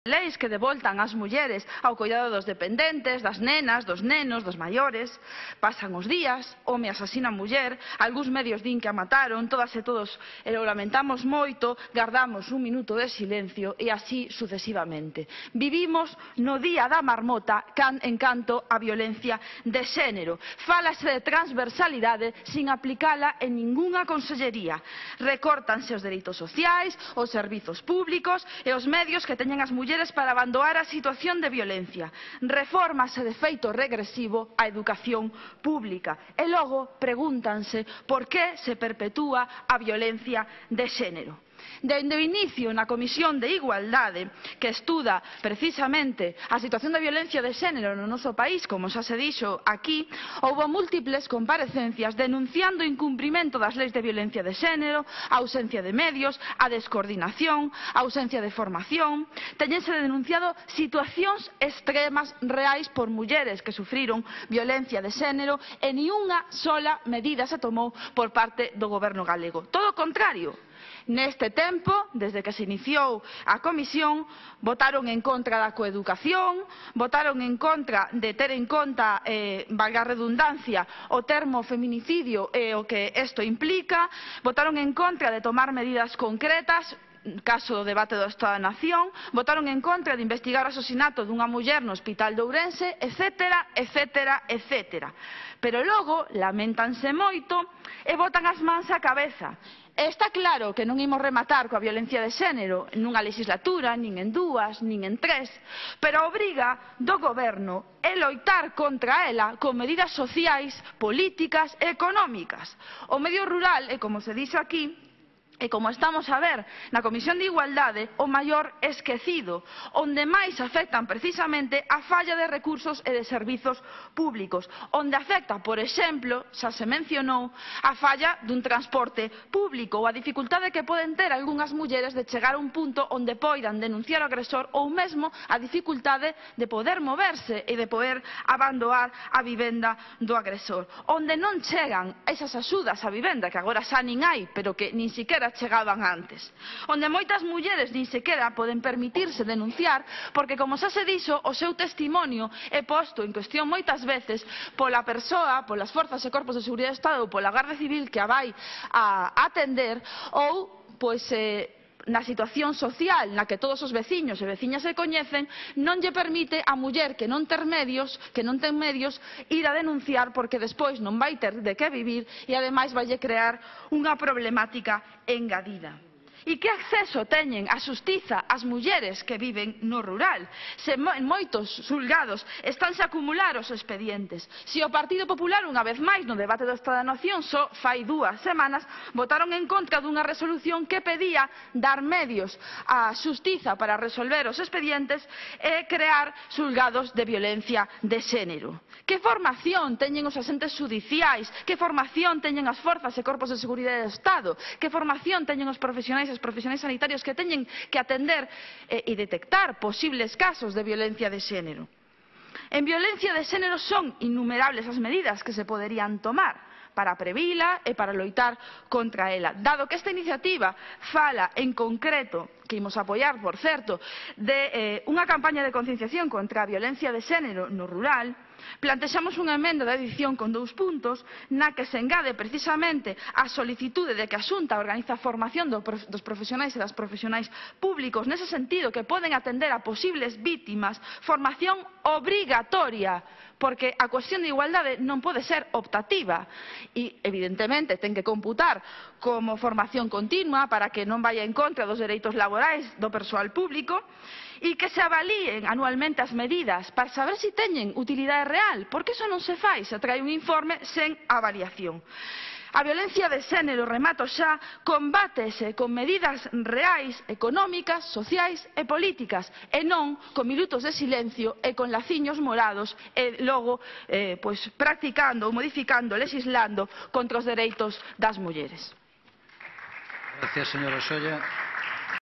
"A violencia de xénero combátese con medidas reais e transversais"
A deputada de AGE, Eva Solla, incide nos problemas das mulleres do rural para loitar contra a violencia de xénero. "A violencia de xénero combátese con medidas reais e transversais, non con minutos de silencio", aclara.